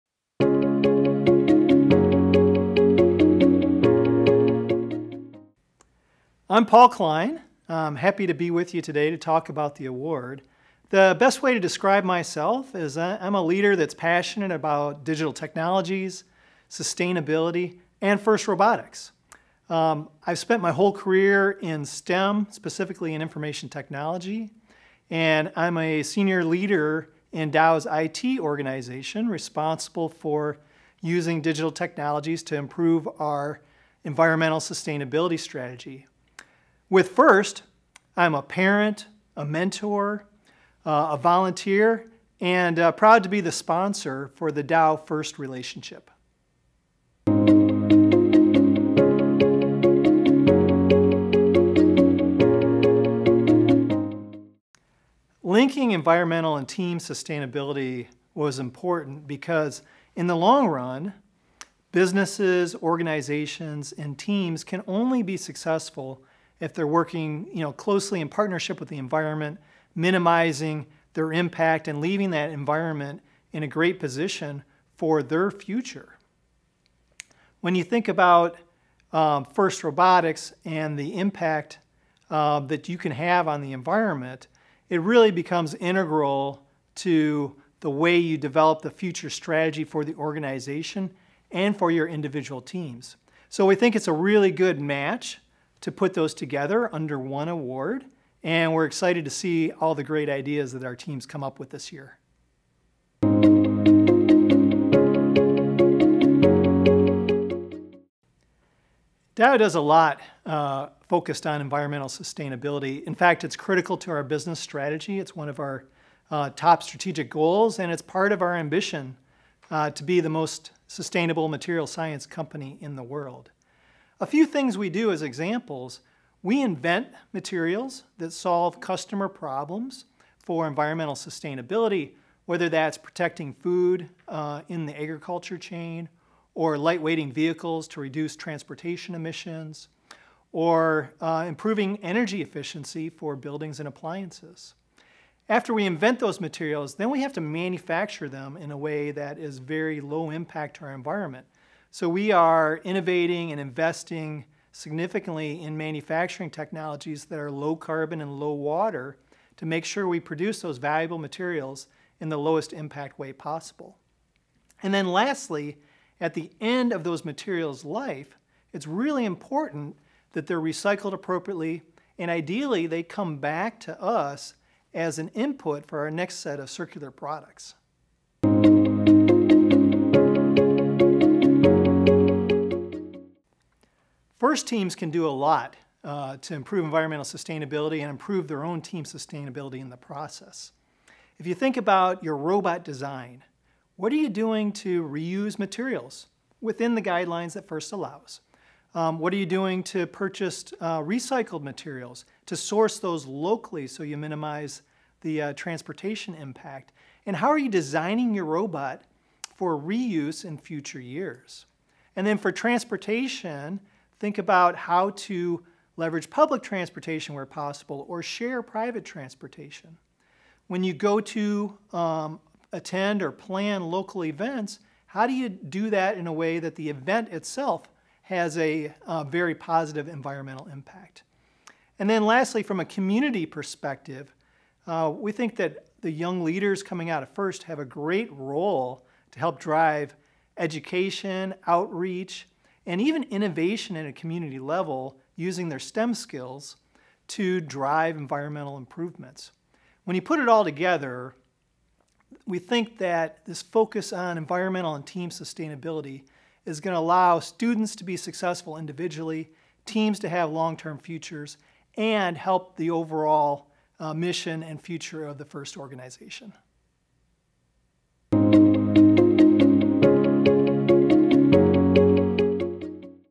FIRST Robotics Competition Sustainability Award sponsored by Dow
We’re so excited to announce that Dow is now the sponsor of the Team Sustainability Award!

To learn more about the Team Sustainability Award presented by Dow,